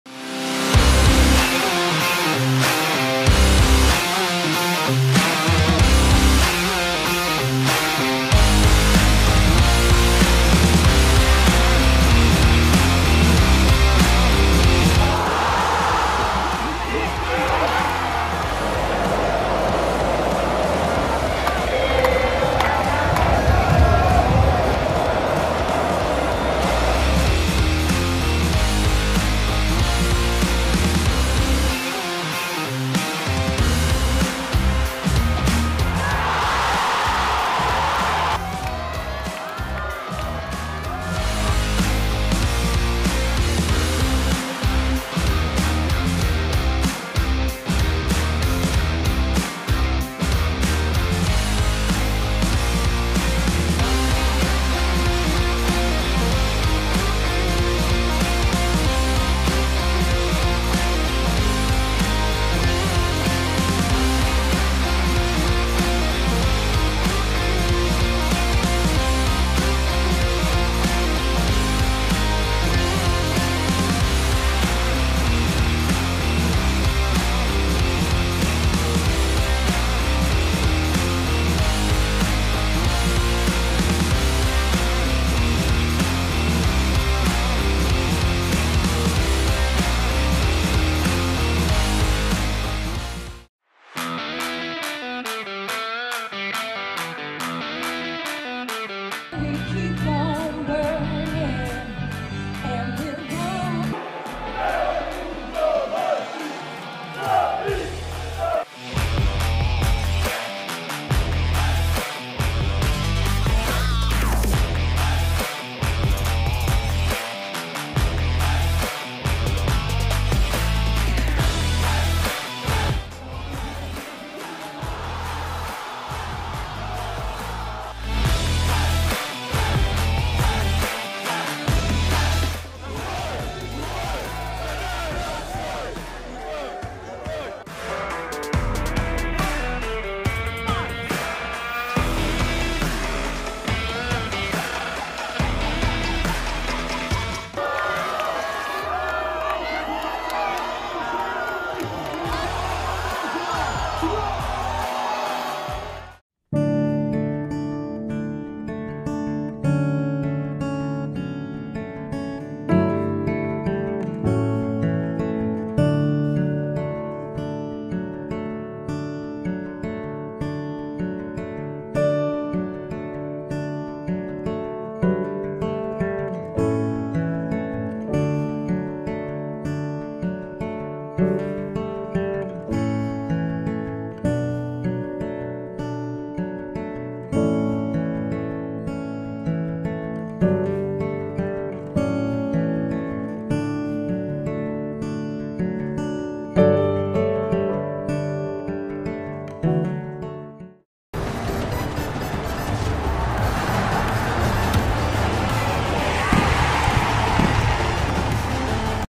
梅西再封神！霸氣創4歷史紀錄：不會讓出獎盃｜世足｜阿根廷｜四強
☑影片授權：路透社